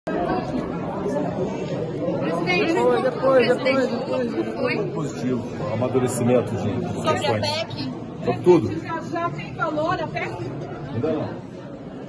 Pacheco diz que encontro com Lula foi "positivo", mas que valor da PEC da Transição não foi definido
O presidente do Senado, Rodrigo Pacheco, disse que o encontro com o presidente eleito, Luiz Inácio Lula da Silva, foi "positivo", mas que ainda não definiram o valor do Orçamento que será destinado à PEC de Transição. O encontro ocorreu nesta quarta (30), em Brasília. Mais cedo, Lula também se encontrou com o presidente da Câmara, deputado Arthur Lira (PP-AL).